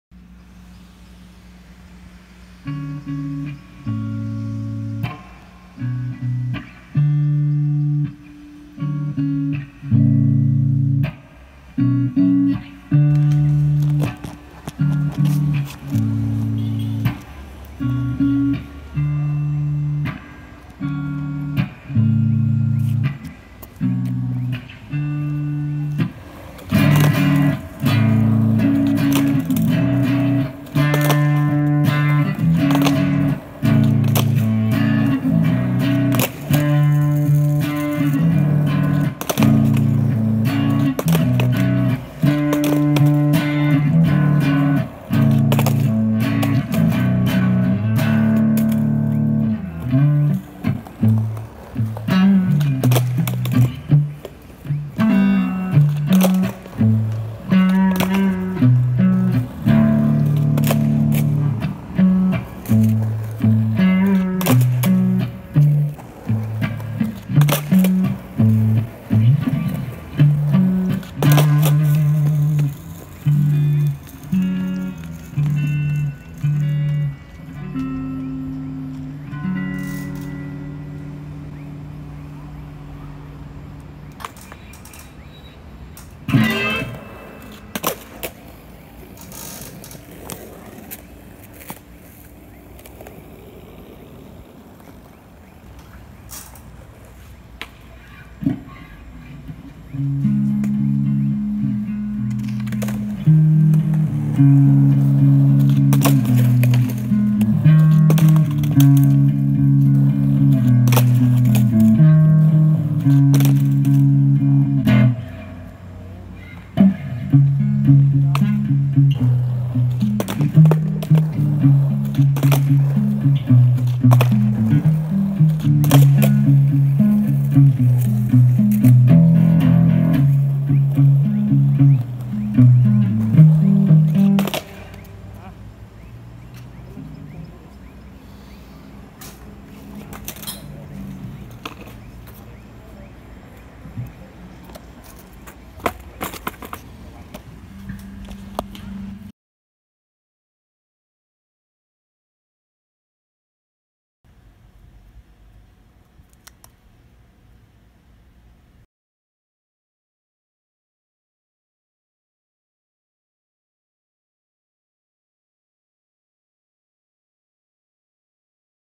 looping sketchy fakie big flips but still no loop pedal
a shorty

gear:
giannini classic t
squier affinity telecaster
mooer radar eq
xotic AC booster
a board